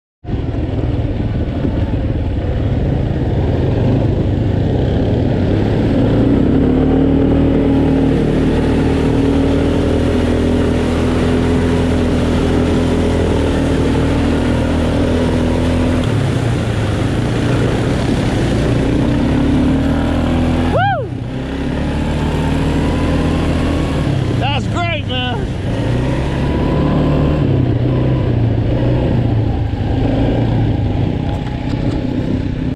ATV riding with GoPro 7 black
Riding in the rainforest of Costa Rica with my new GoPro 7 black attached to the chest accessory.